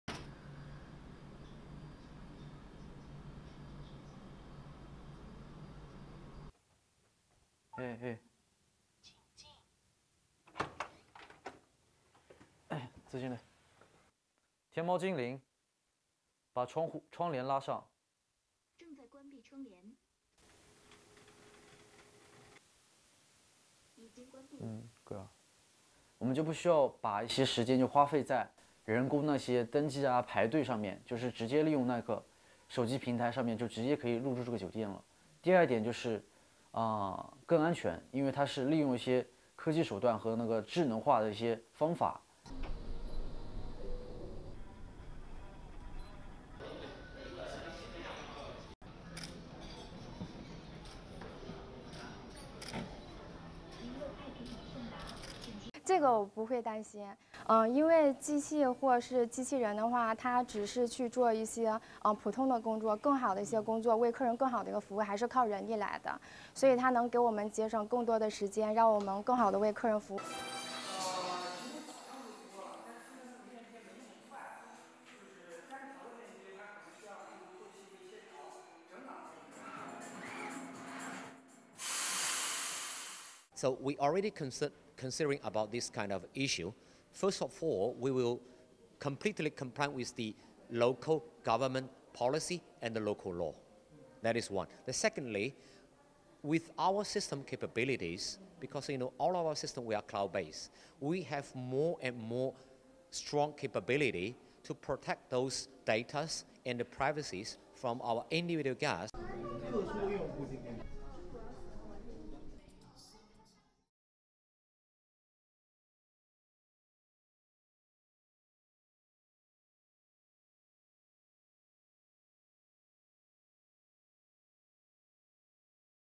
Alibaba Opens AI "Future Hotel"
Chinese internet giant Alibaba has opened a hotel loaded with artificial intelligence (AI) and robots, automating a series of procedures like check-in, lights control and room service.